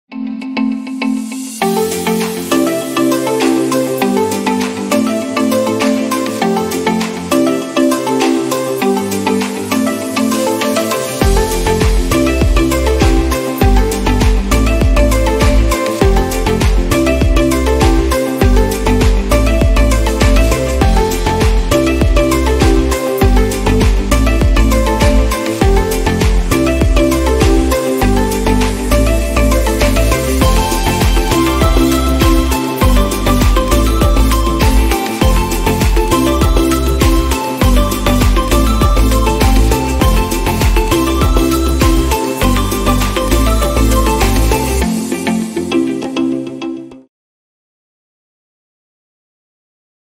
Exhibition on Favorite Places of UrFU Students
"I Love My Homeland! I love Russia! I love Ekaterinburg! I love UrFU!" photo exhibition opened in the main academic building of Ural Federal University.
UrFU students from more than 30 countries presented their favorite places in their home countries and Ekaterinburg.